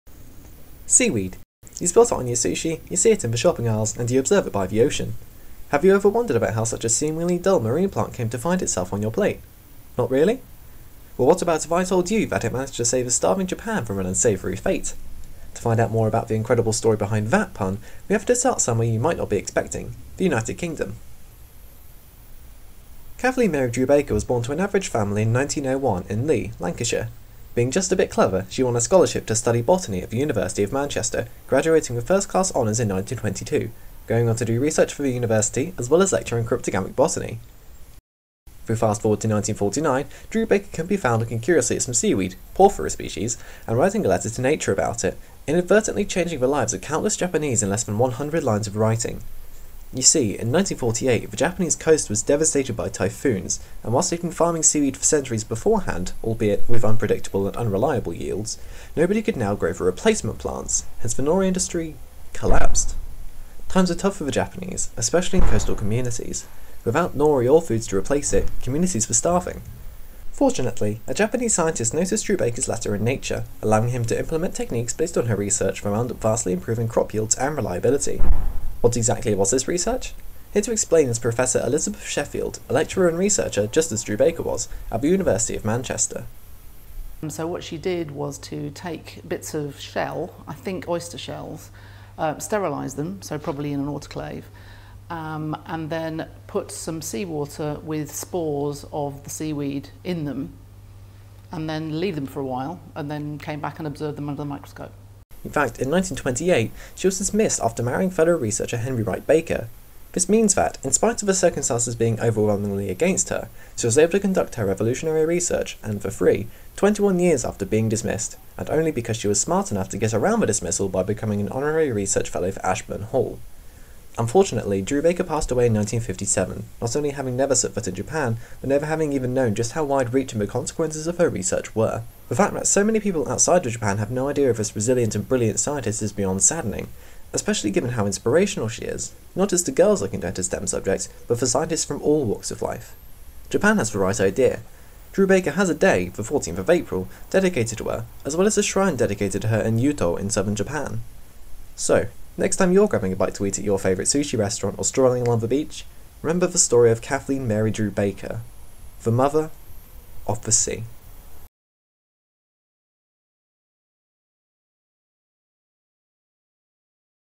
Kathleen Mary Drew-Baker & Seaweed (Student Film Competition)
Kathleen Mary Drew-Baker was an inspirational figure in the 1940s, when she revolutionised the Japanese Nori industry by conducting research into the life cycle of the seaweed Porphyra laciniata. This was published in the journal Nature, transforming the lives of many Japanese people who were unable to harvest seaweed, after their plantation beds were destroyed by a series of typhoons. Even today she continues to have a profound impact; from being recognised in Japan with her own national day, serving as a symbol for gender equality in the STEM subjects, and allowing the world to enjoy a tasty and readily available food: sushi!

Filmed & produced by Alex Taylor, Stephanie Pruden, Rachel Thomson & Florence Costalas, The University of Manchester.